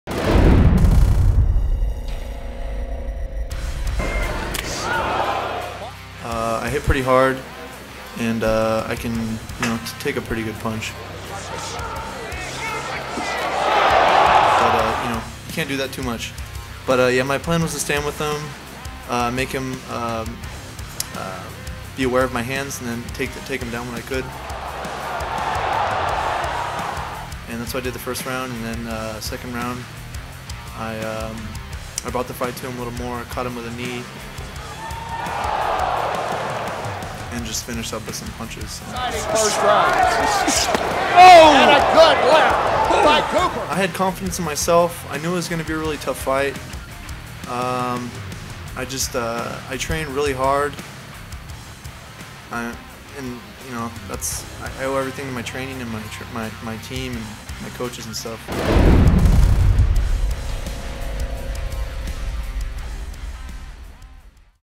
Brett Cooper defeats Rory Markham Interview
Cooper starts fast, landing several good left hands that sur